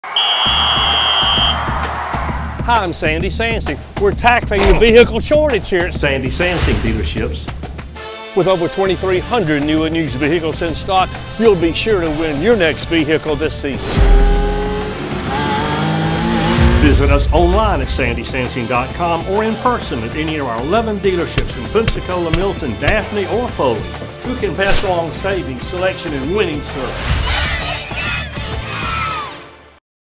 Football Season is Here and so are the Deals at Sandy Sansing Dealerships!
Football season is here and we are as excited as you are so we want to PASS along Savings, Selection, and Winning Service to you!!! today!